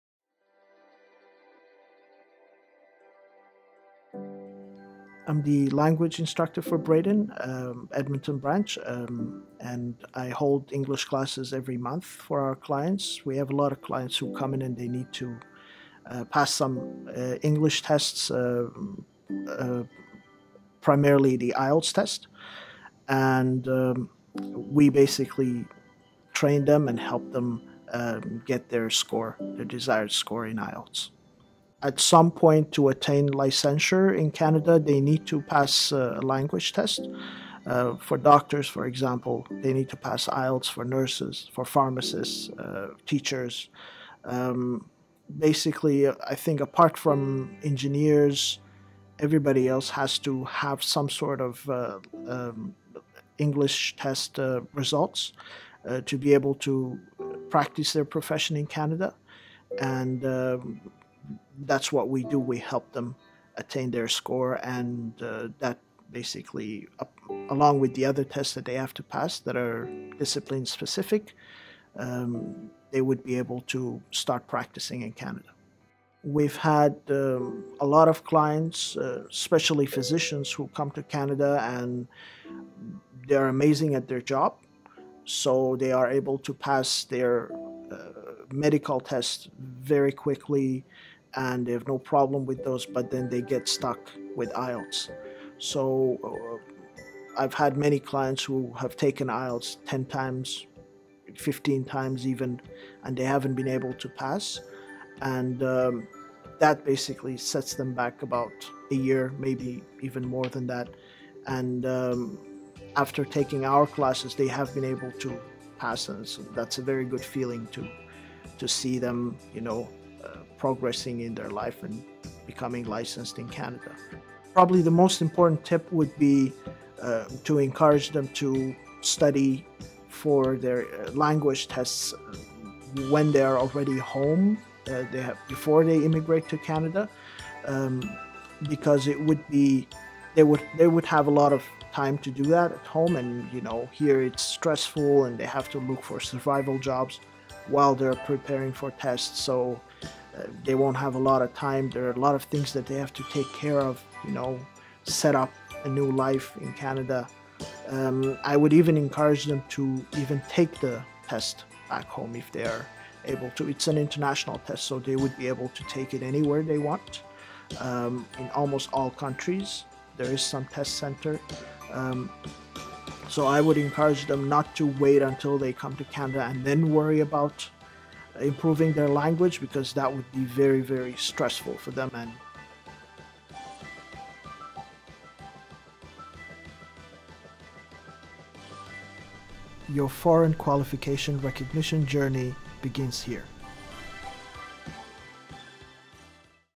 Language requirements in Canada
Depending on your profession you may be required to take a language examination to enter your profession in Canada. Kara, Bredin Centre for learning, English Instructor explains some of those exams, difficulties faced by some of our clients and some tips to get you started.

Bredin Centre for Learning offers a list of programs to aid you in finding employment in Alberta. to learn more.